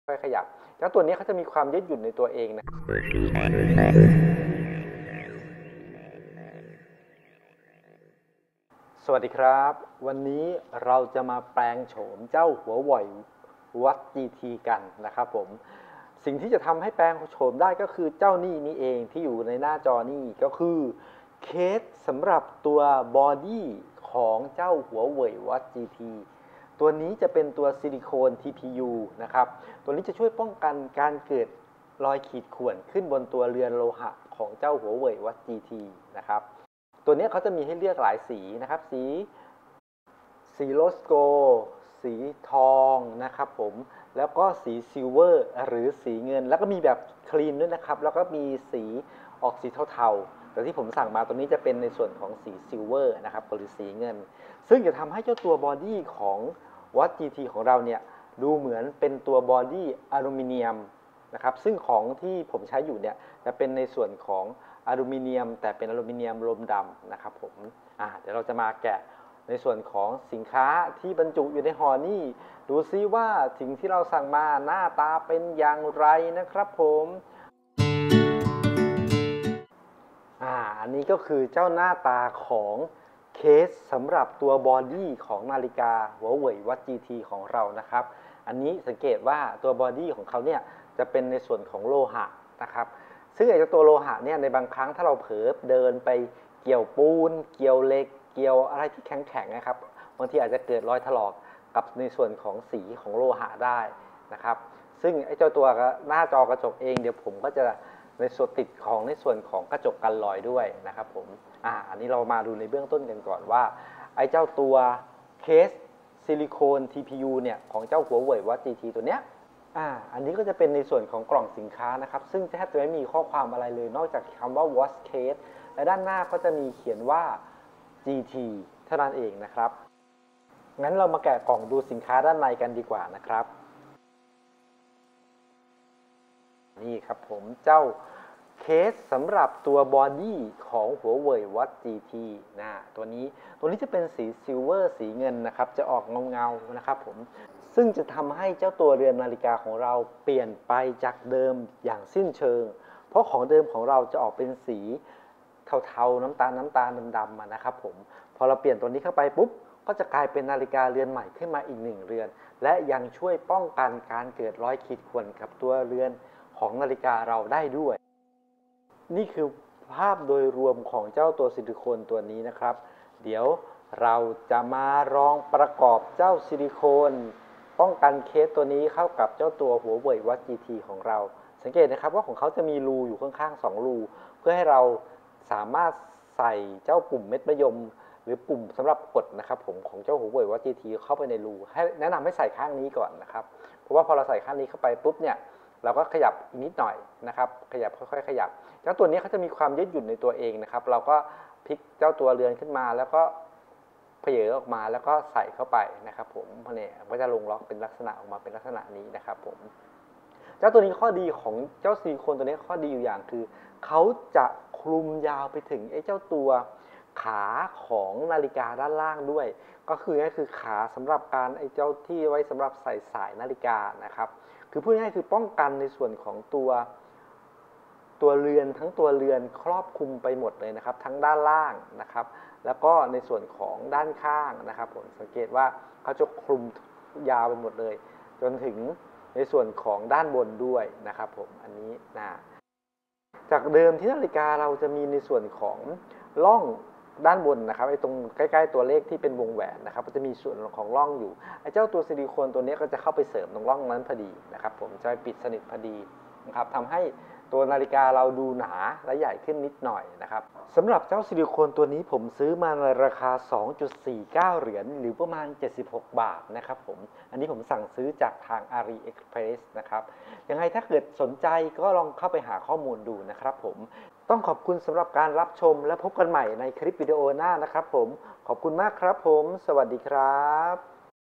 Review Case Huawei watch GT Active Silicone TPU Silver
แปลงโฉม Huawei Watch GT Active ด้วย Silicone ป้องกันรอยตัวเรือนสีเงิน
ปัจจุบันราคาอยู่ที่ 44 บาท
เผื่อใครกำลังมองหาสินค้าชิ้นนี้อยู่ สามารถหาซื้อ และตรวจสอบราคาได้ตาม link ด้านล่างนะครับ

รวมสินค้าแท้ลดราคา เอามาฝากครับ